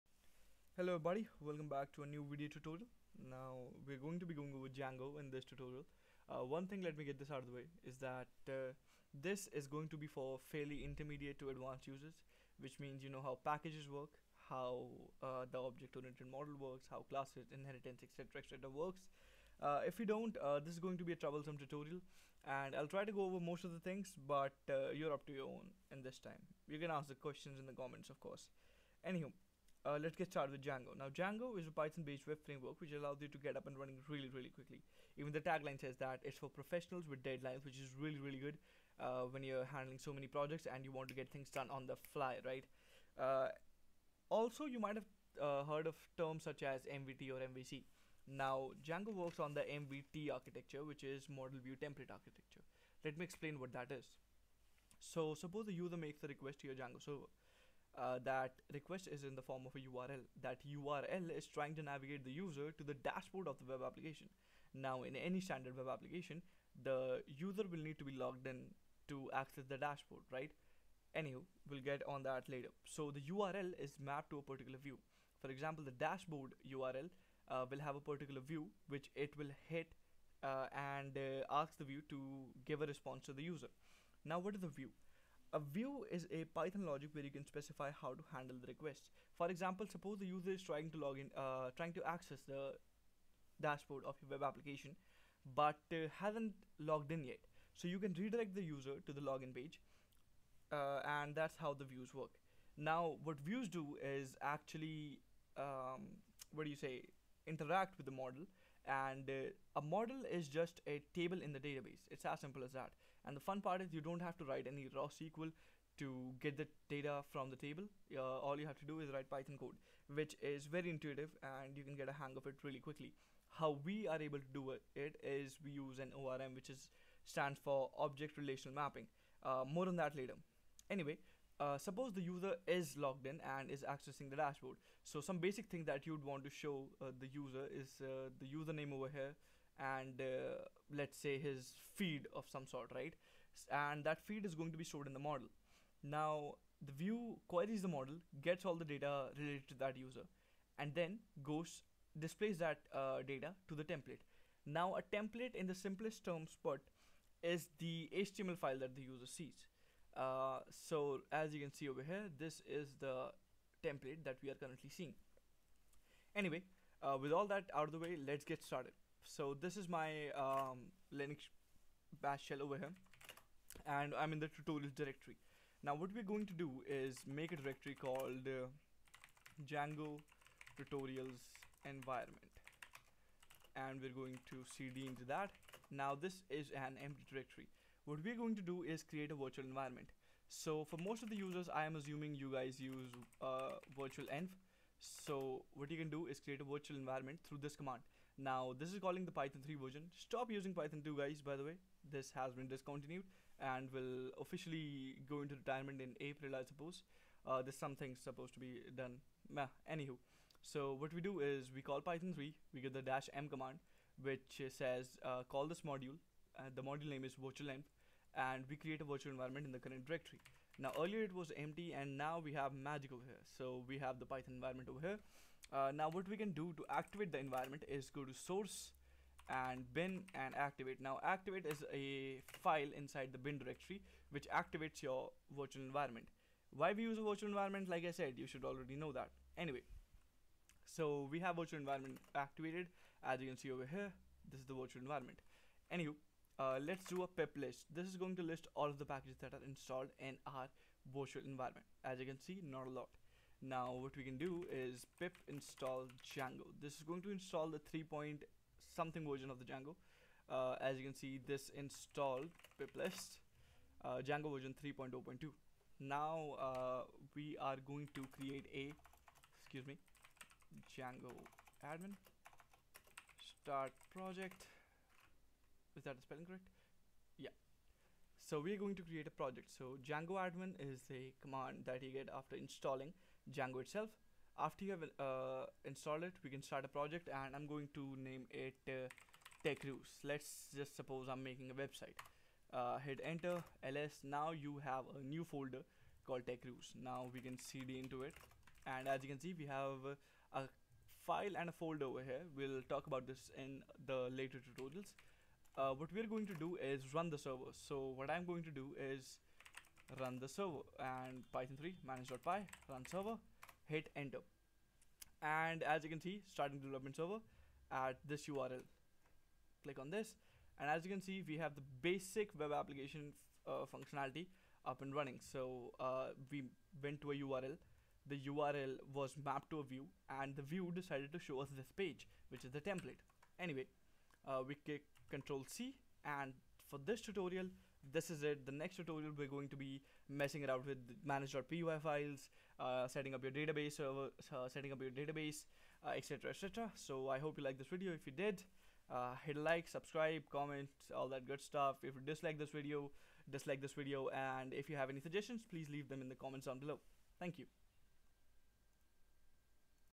Advanced Django 3.0 Tutorials - Introduction and Installation
Welcome to Techruse, this video contains explanation of basic Django architecture and virtualenv setup along with installation and first server run.